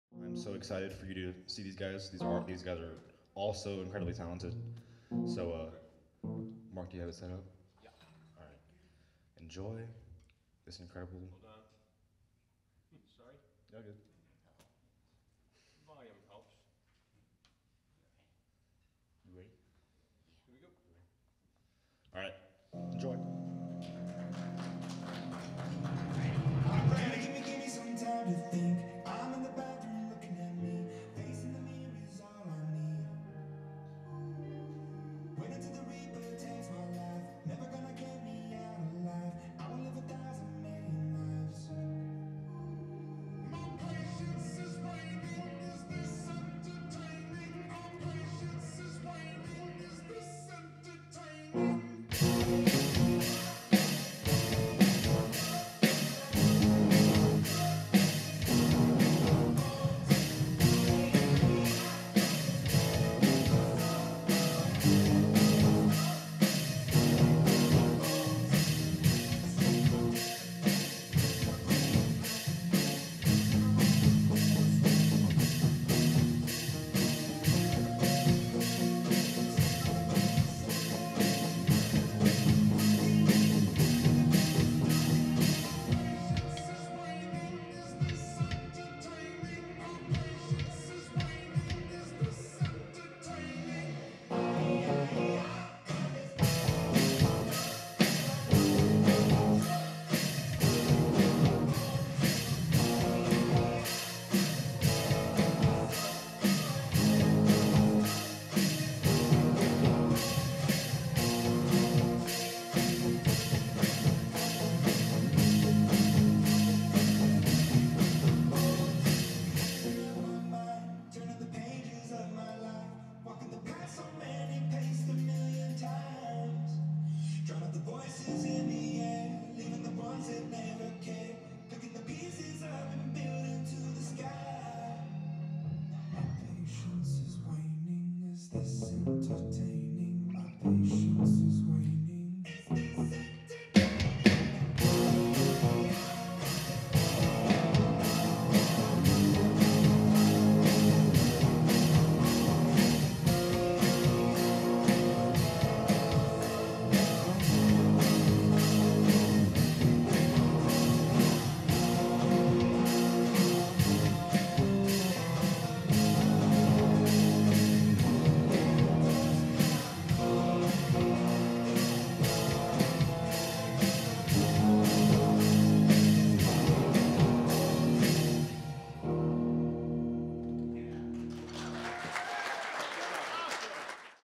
Adventures in Music's Spring Recital 2023 Danny and Jace Band
On April 1st, 2023, students of Adventures in Music performed their well-practiced music for the Spring Recital!